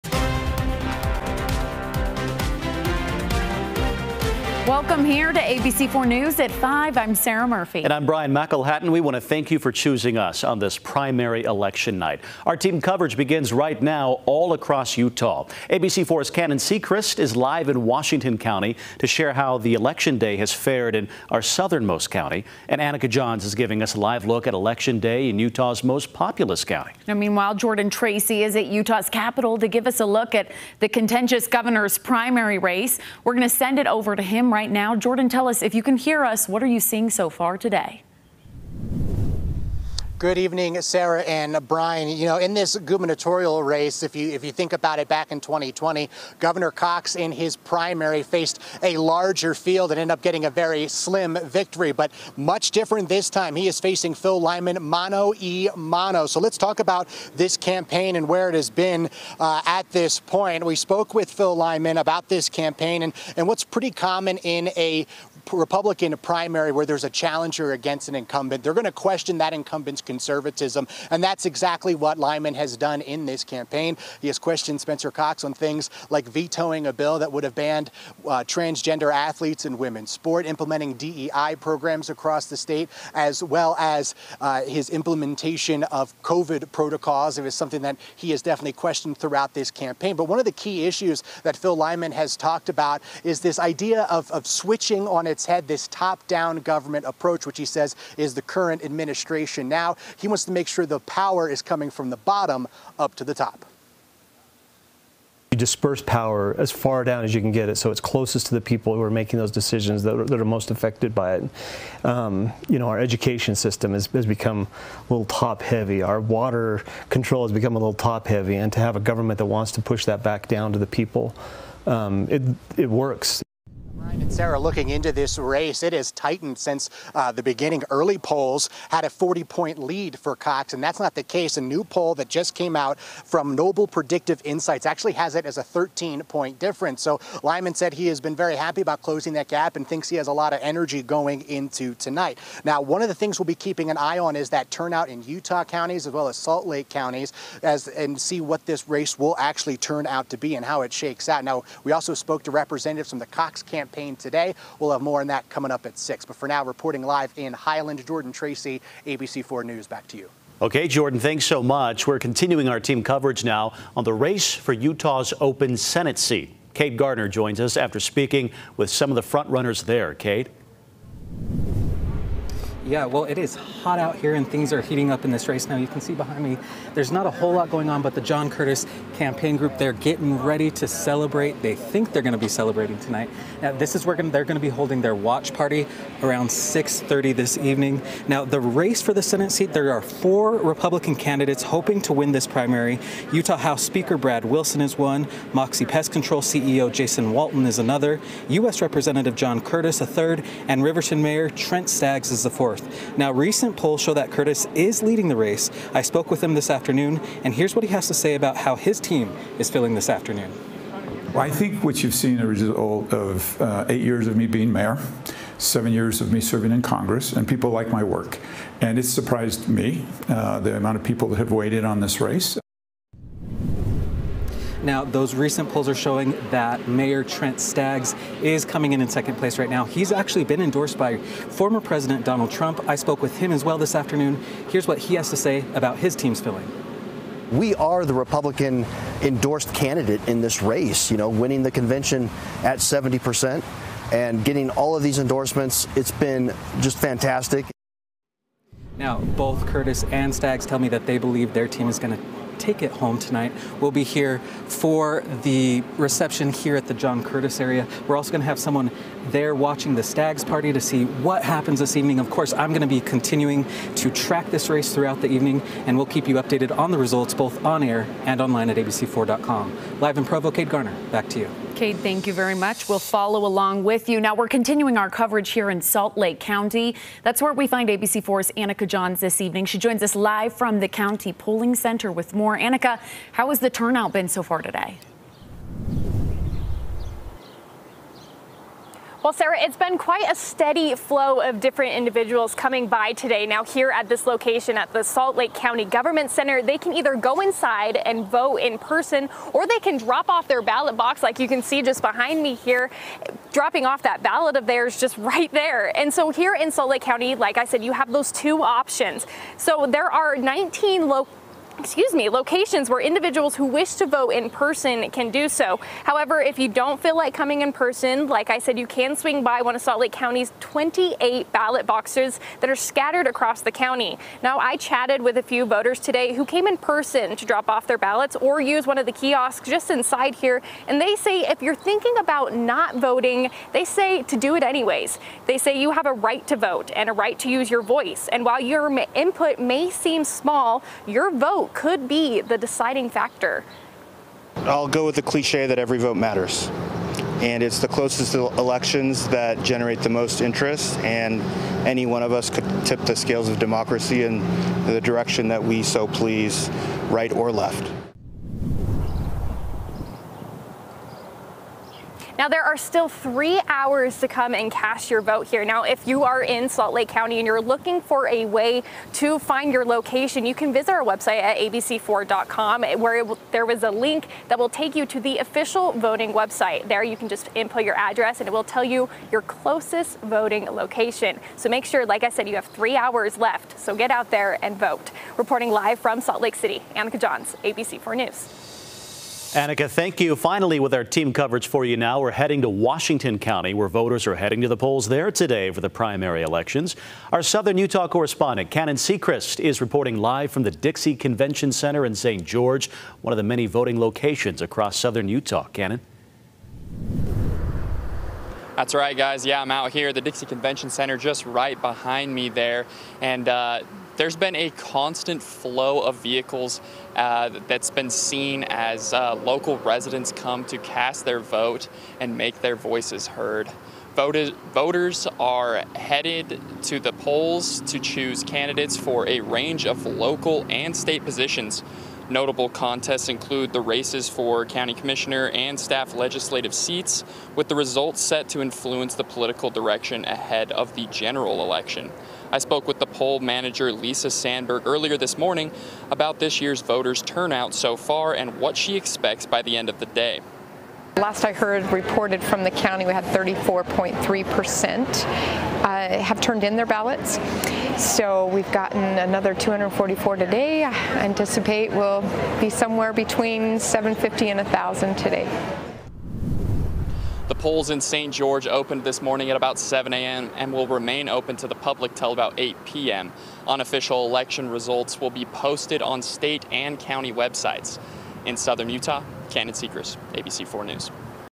Team coverage on Utah Republican primaries -- 5 p.m. update
It's primary election night. Here is a look at election day from across the state of Utah.